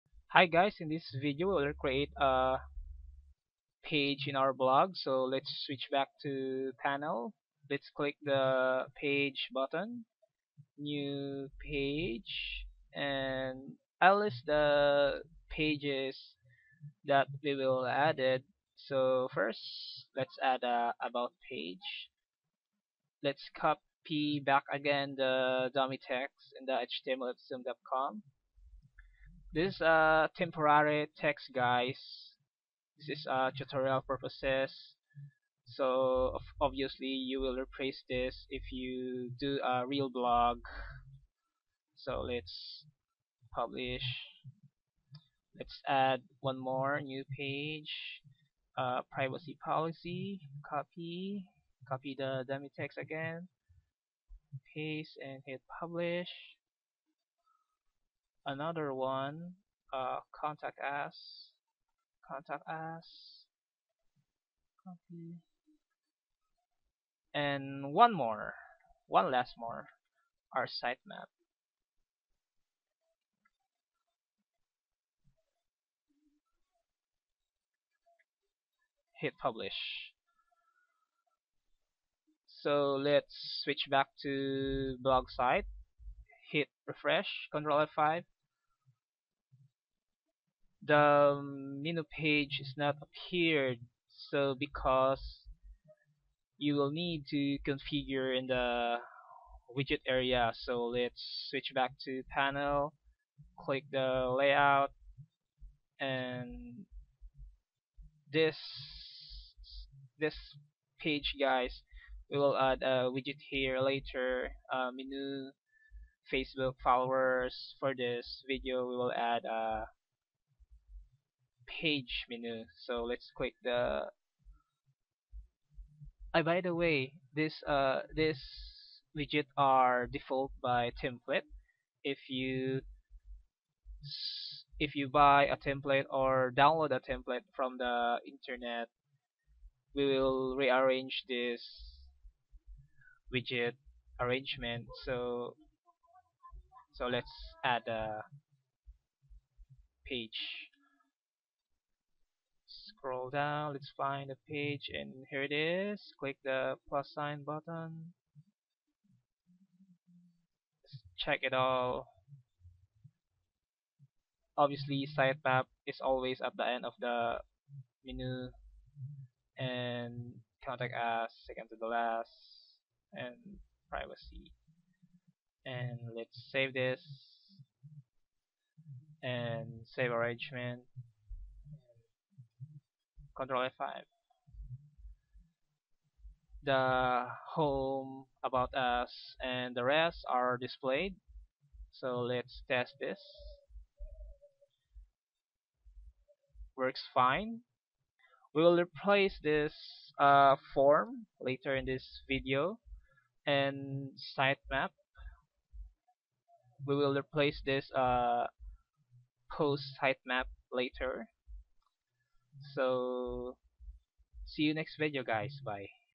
Blogger Tutorial for Beginners - How to Add Pages or Tabs - Part 5
In this part 5 of blogger tutorial for beginners 2016 series, we will add pages in our Blog, About us, Contact us, Privacy Policy and Sitemap Page, we will put a dummy text at this moment, but will update the content of contact us and sitemap page in future video.

This video will continue on part 6...

PRODUCED BY:
Axl Mulat